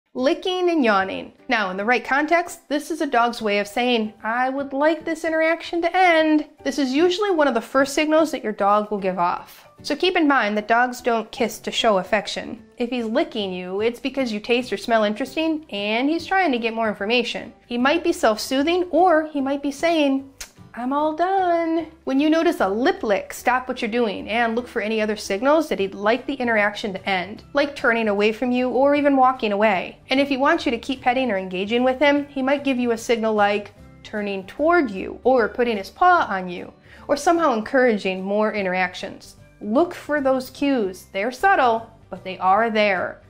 Dog Body Language What to Look for onlinedogtraining
What is the best way to tell a dog is stressed? How well do you understand dog body language? We are your canine communication experts!